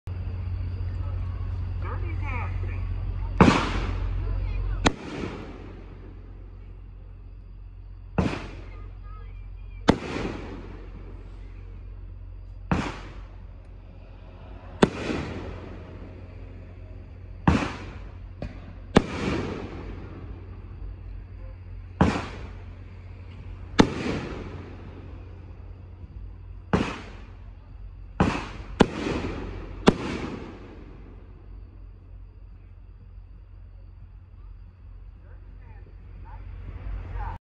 Dirty Dancing in the sky - Showtime
This cake has good big breaks but it’s only 7 shots so that kind of sucks!